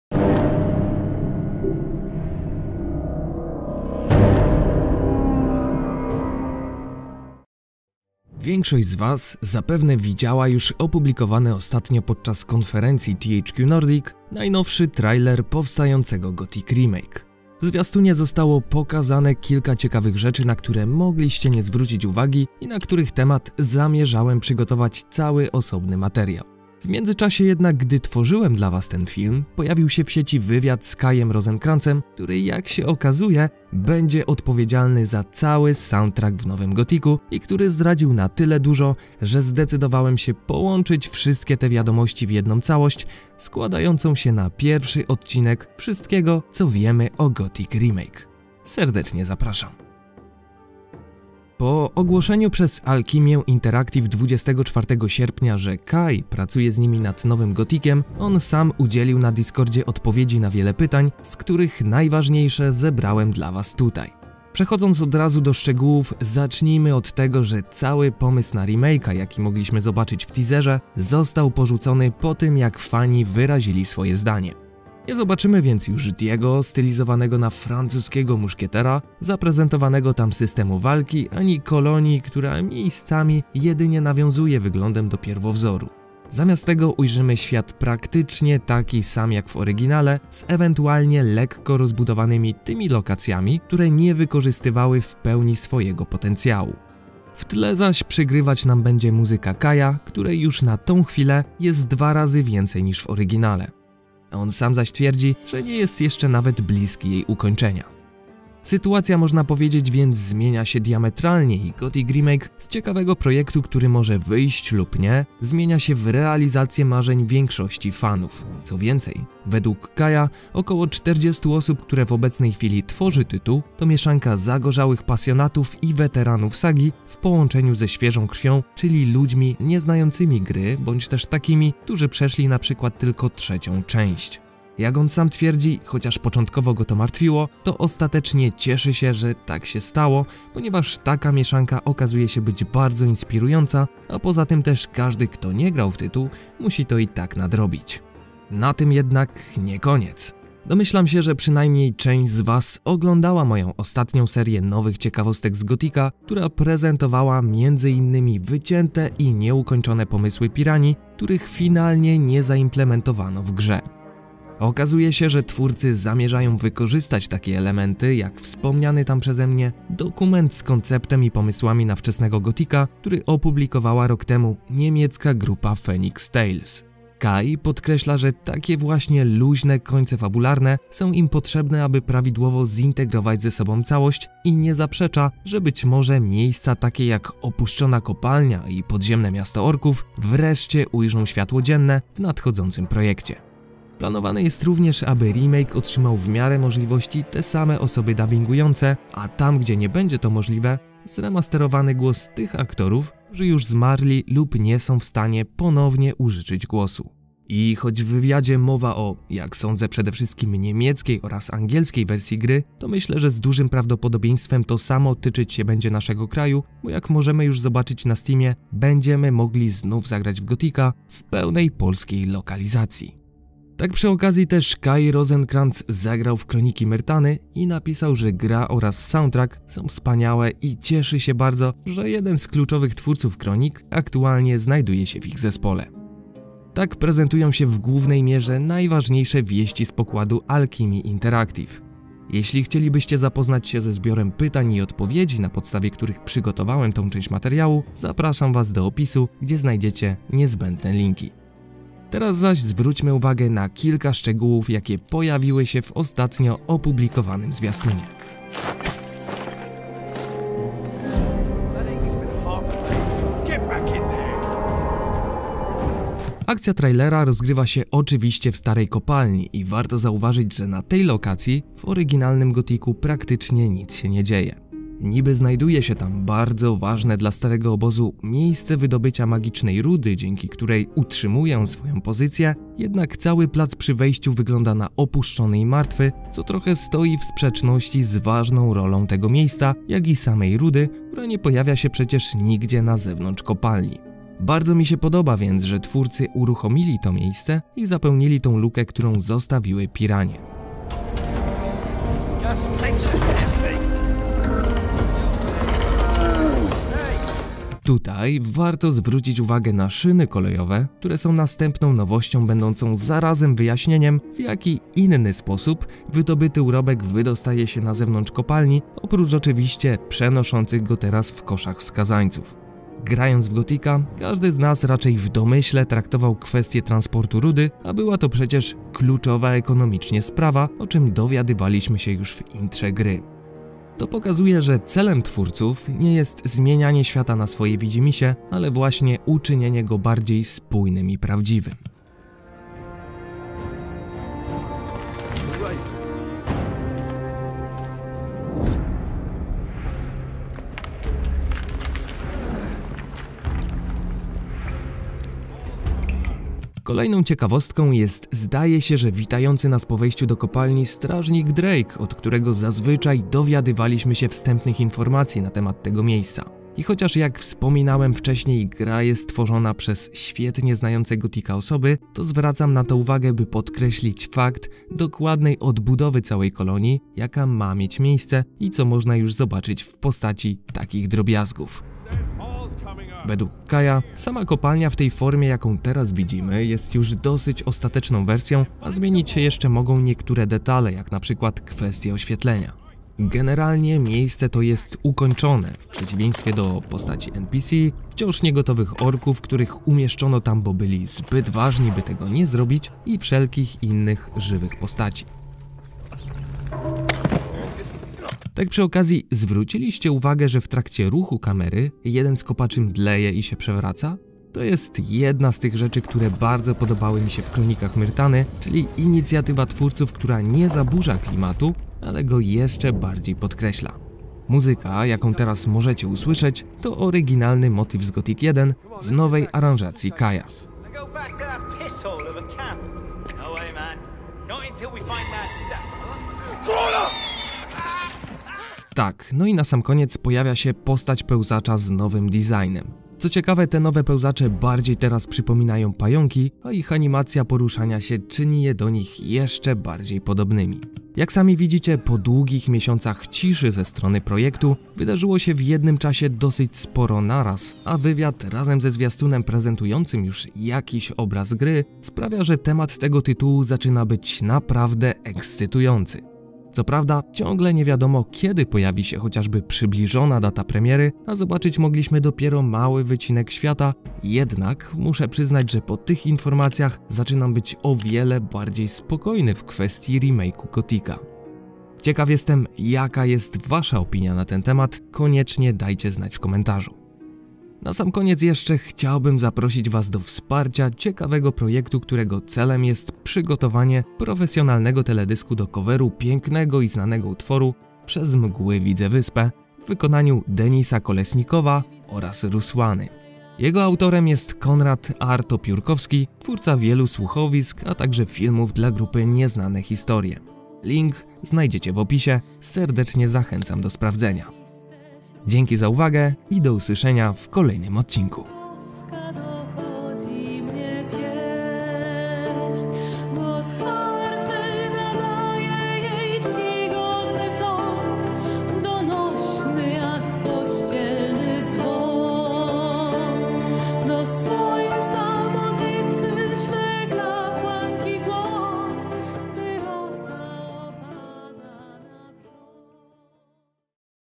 Wszystko co wiemy o Gothic Remake cz.1
Po długich miesiącach ciszy, wypełnianych jedynie bardzo szczątkowymi informacjami o projekcie Gothic Remake - nadszedł wreszcie moment gdy właściwie za jednym zamachem dowiedzieliśmy się naprawdę sporo. I trzeba przyznać, że jest dobrze a zapowiada się jeszcze lepiej. Serdecznie zapraszam do materiału!

💎 Chciałbyś mi pomóc w rozwoju? Dzięki!
💎 Zajrzyj na fanpage by wiedzieć o wszystkim co aktualnie robię z wyprzedzeniem